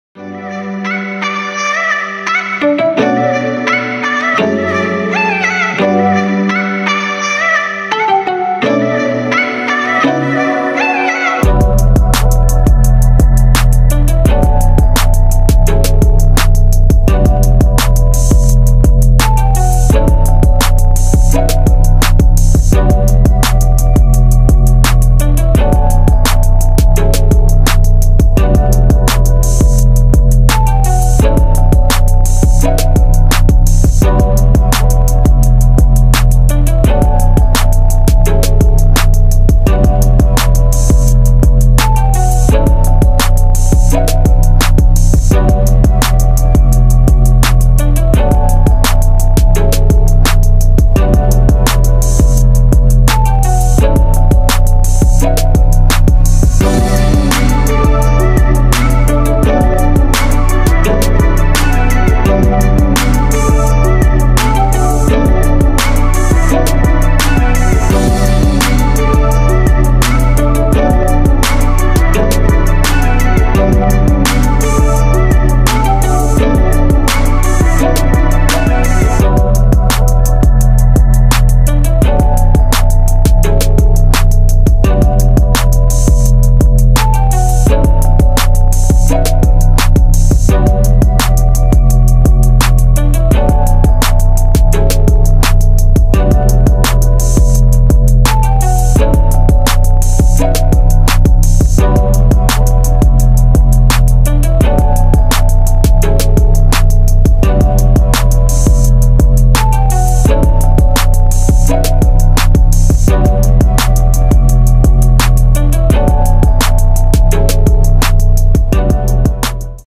Wake boarder gets a free video
Flying my Mini Pro 3 drone and captured a wake boarder carving up the water off Hollywell, Queensland. The video says it all...